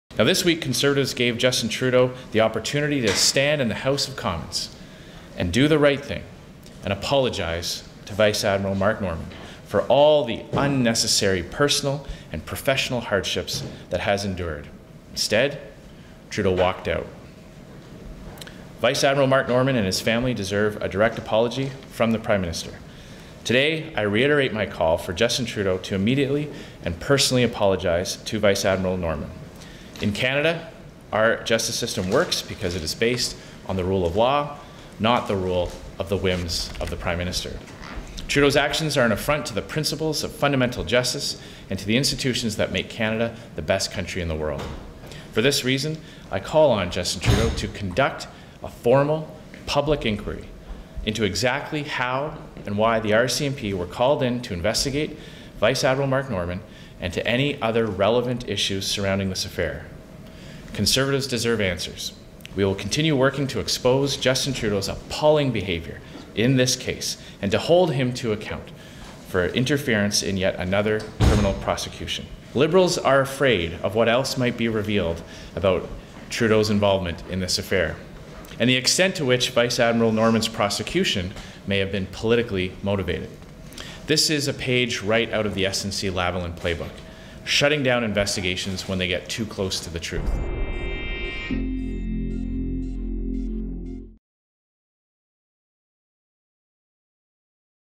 Mark Norman affair needs public inquiry, Scheer says
Conservative Leader Andrew Scheer says Canada needs a public inquiry into the case of Vice-Admiral Mark Norman. On Parliament Hill Friday, Scheer says a full investigation is justified to examine how the military's former second-in-command came to be charged with over alleged leaks of cabinet secrets about a shipbuilding contract. The case against Norman collapsed last week after Crown prosecutors got new information from the defence that convinced them they stood no reasonable chance of landing a conviction. (May 17, 2019.)